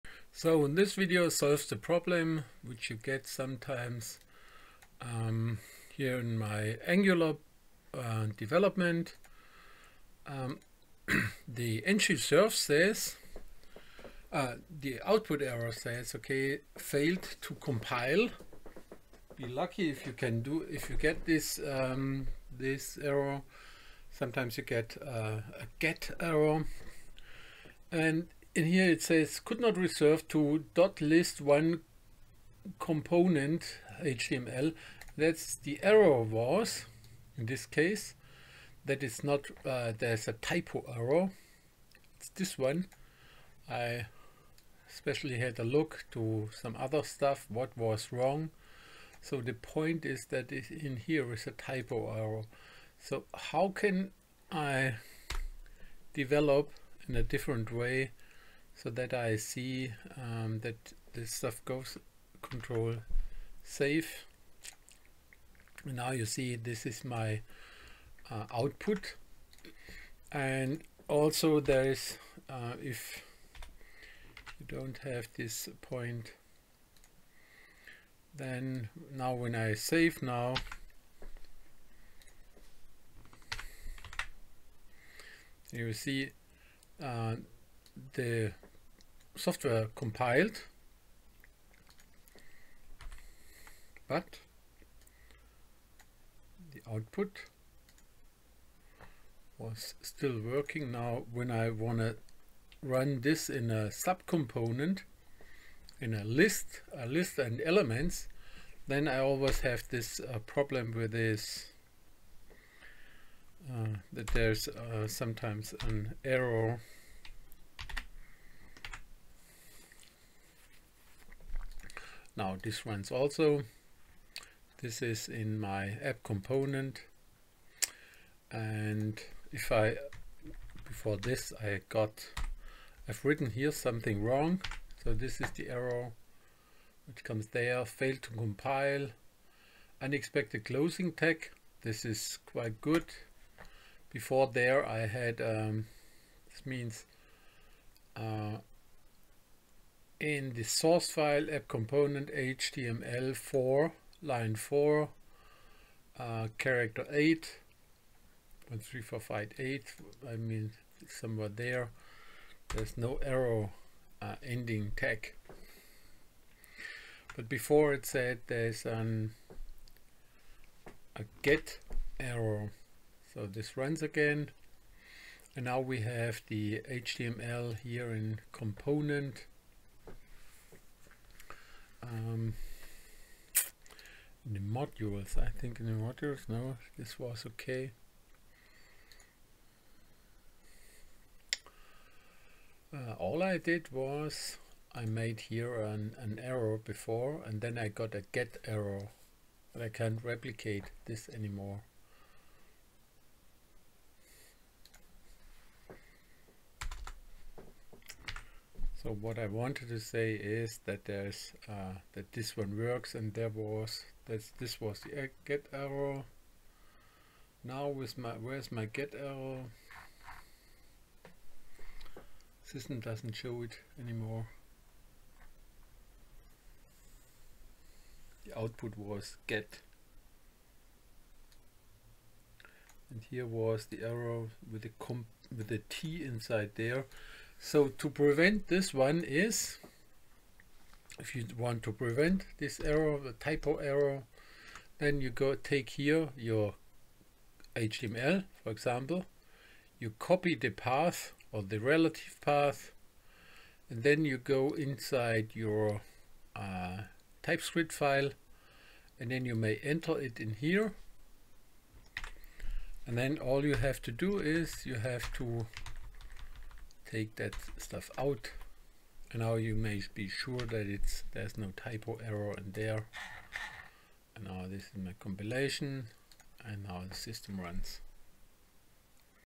Solved Angular Error: HostResourceResolver: could not resolve component.html
Angular Fehler:  Error: HostResourceResolver: could not resolve

Fehlermeldung:

Error: HostResourceResolver: could not resolve list1.comptonent.html in context of D:/Programmierung/Angular/Kurs/myapp02/src/app/components/list1/list1.component.ts)

Lösung:
Es handelt sich um einen Tipp-Fehler in der component.ts Datei, im welcer der Pfad auf die component.html Datei falsch geschrieben wurde.
Man kann den Fehler übergehen, indem man den Pfad im Baum kopiert und einfügt

Fehlermeldung im Browser in Anguler localhost:4200
Failed to compile.

HostResourceResolver: could not resolve list1.comptonent.html in context of D:/Programmierung/Angular/Kurs/myapp02/src/app/components/list1/list1.component.ts)

Lösung zum Vermeiden von Tippfehlern TypoErrors